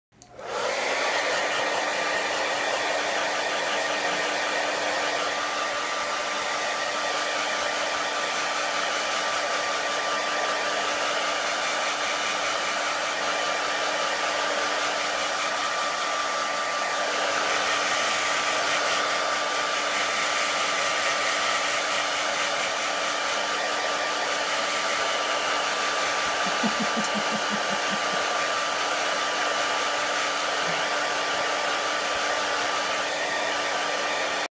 Mini schnauzer doesn't mind getting blow dried
My mini schnauzer Miggy likes getting blow dried after a bath.